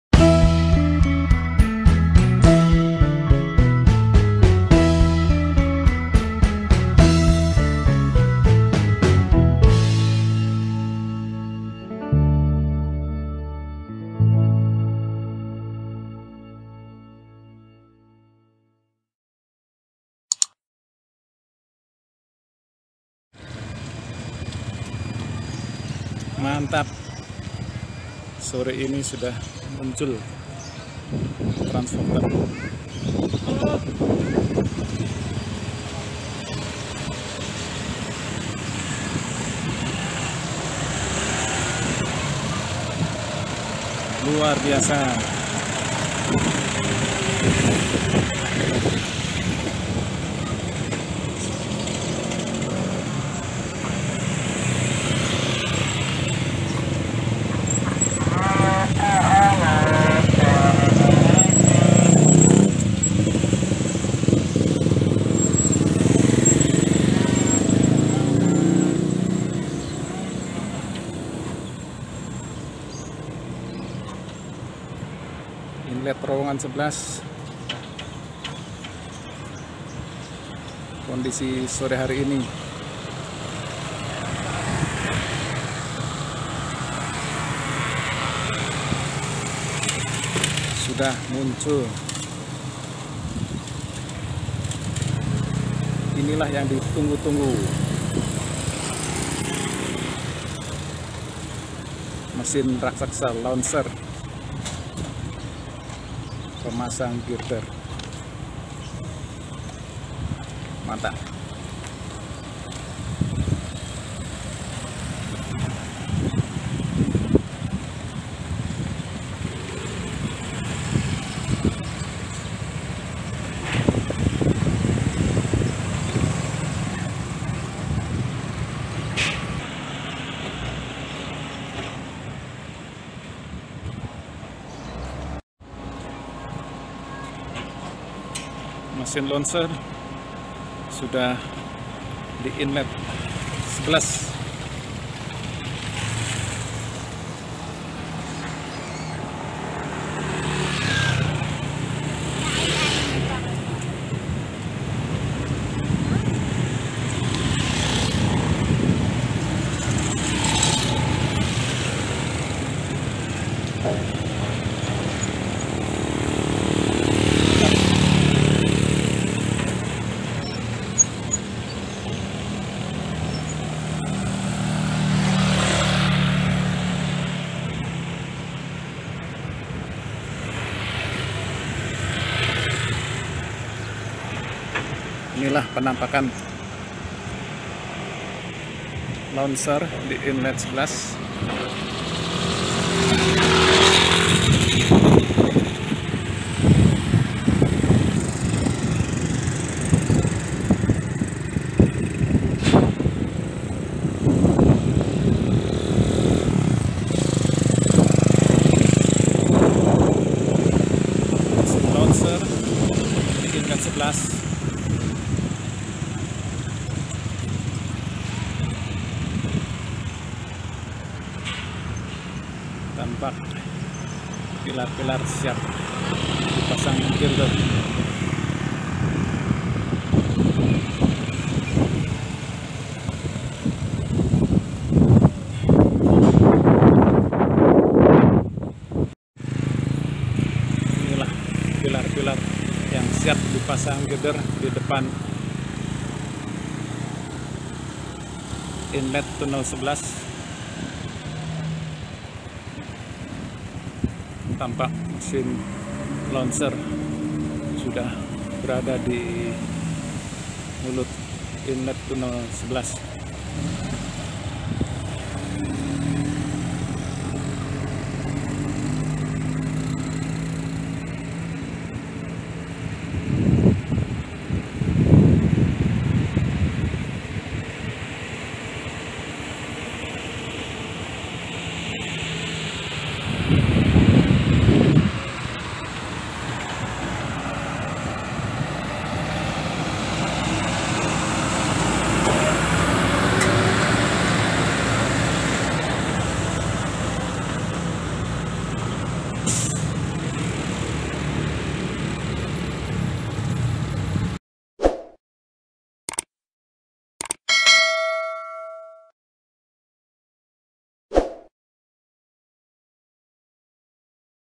LAUNCHER MUNCUL DI INLET TUNNEL11 GUNUNG BOHONG progress KCIC 27 Juli 2021
Tayangan kali ini yaitu Mesin Monster Launcher telah muncul di Inlet Tunnel 11 Gunung Bohong dengan demikian penginstalan girder2 kearah Tunnel 10 Padalarang akan segera dikerjakan, semoga semuanya berjalan lancar tanpa hambatan
Tetap semangat dan jaga kesehatan
tak lupa tetap Prokes
Salam Sukses